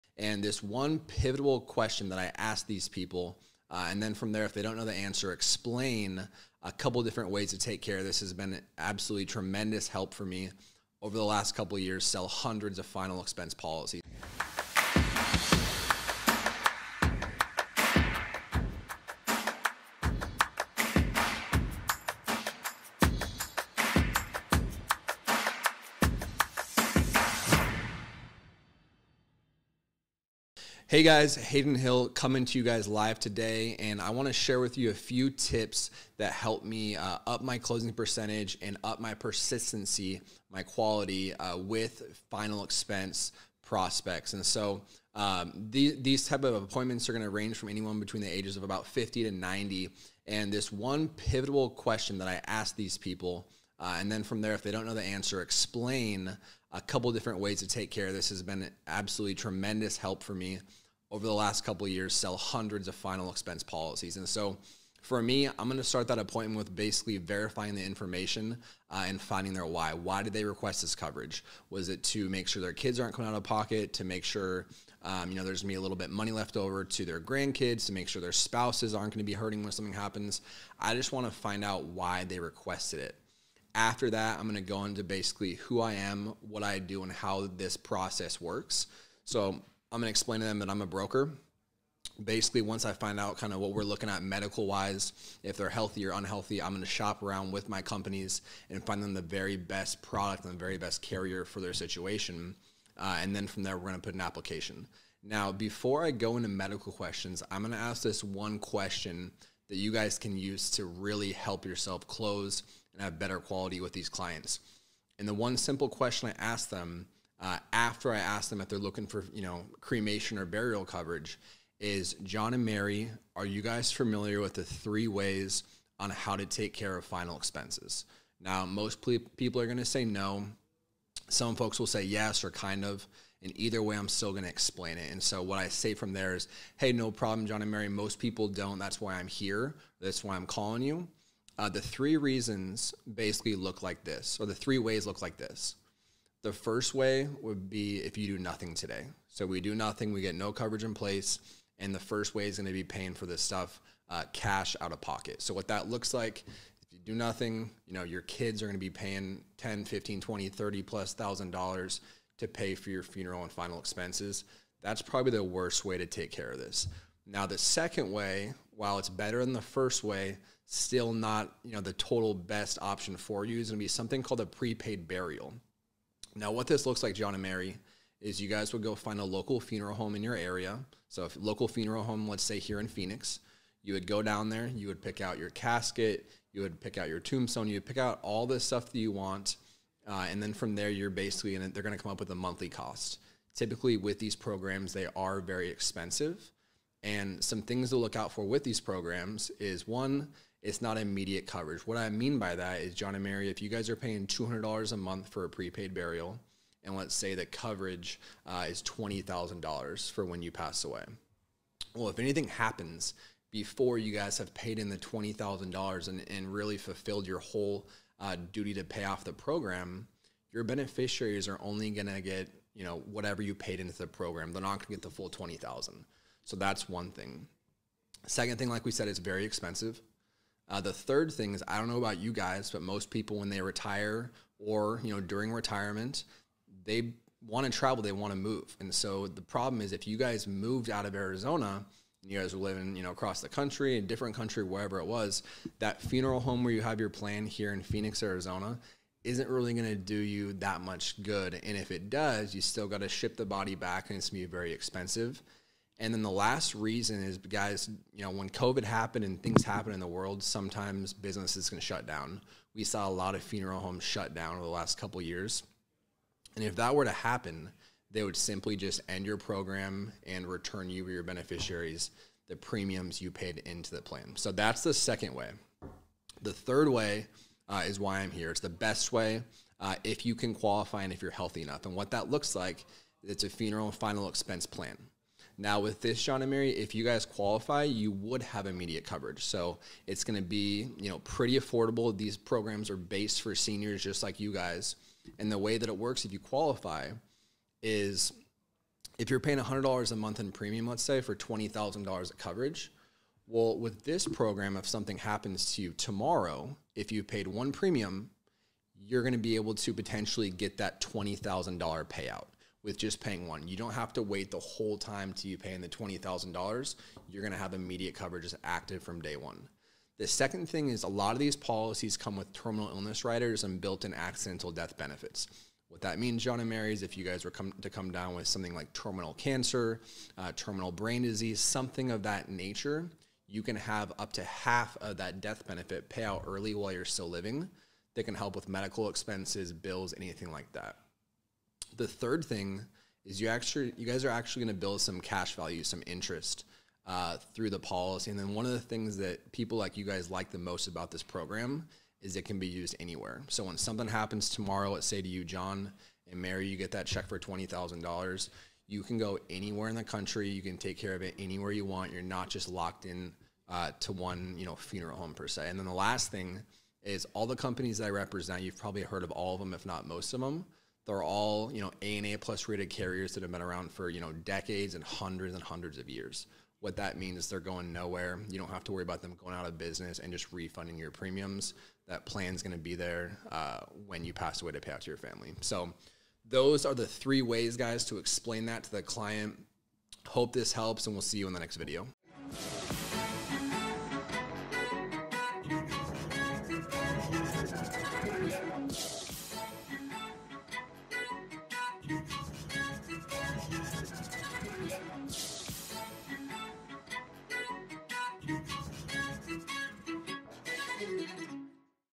The 3 Best Ways To Explain Final Expense Insurance Options
Integrity Partner, Hayden Hill, teaches you how to present the 3 different final expense options and the #1 question he asks to successfully close the appointment.